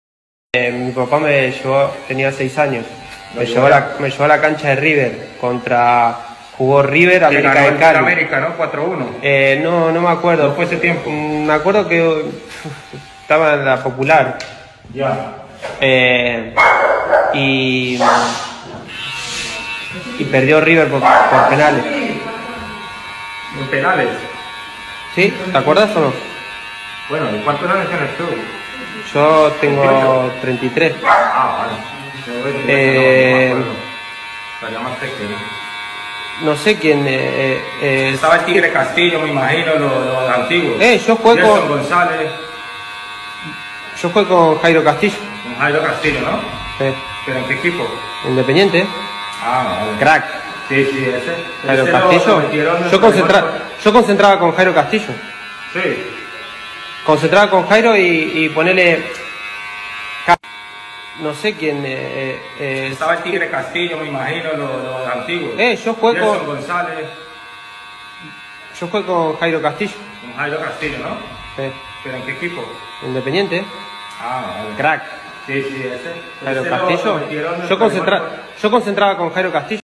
🚨GRANDE💥 EL KUN AGUERO HABLANDO DE AMERICA Y EL TIGRE CASTILLO SABE🔥
🚨🚨el kun sabes hablando de América y su concentración con el tigre castillo! sabe🔥💥🚨